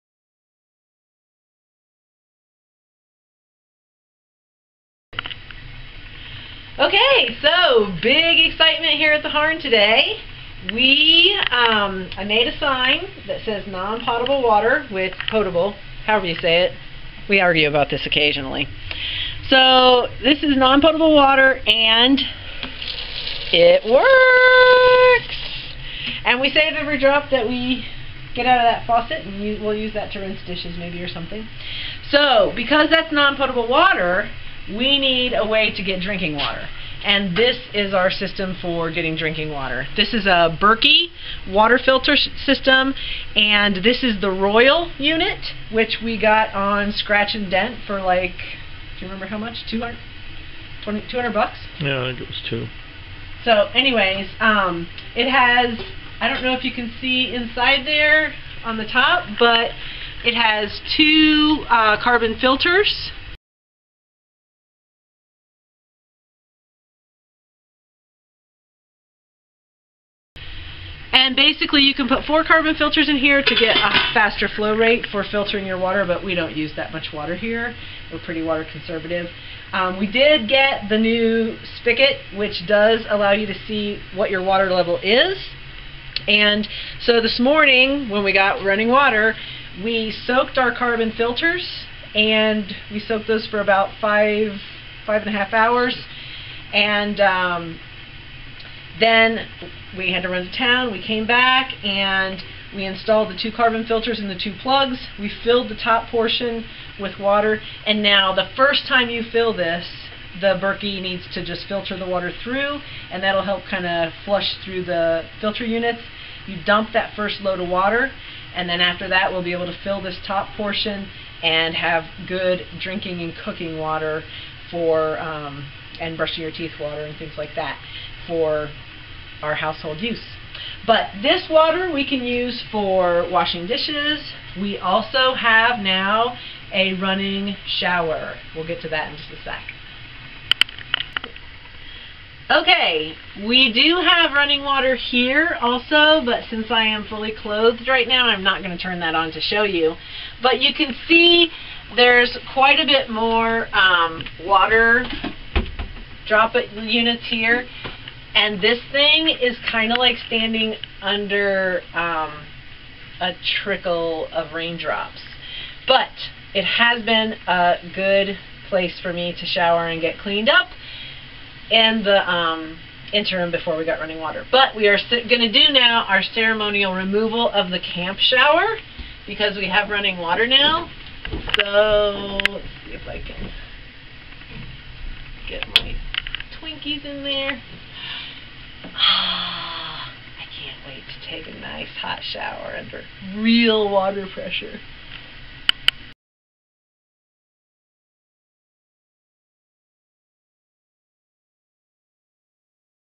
Running Water 2 10 17
The Harn now has RUNNING WATER!  I tell you, when you have to haul in all your water and rely on a trickly camp shower and you can't just push on the tap to rinse your hands at the sink, you REALLY appreciate when you get running water.  Our plumber did a great job installing this system, which was atypical of the normal water systems he normally installs. We will have a video of the installation shortly.
UPDATE 2-23-17 After having the water tested, we have found it to be free of nitrates and bacteria.  it's Potable!! :-)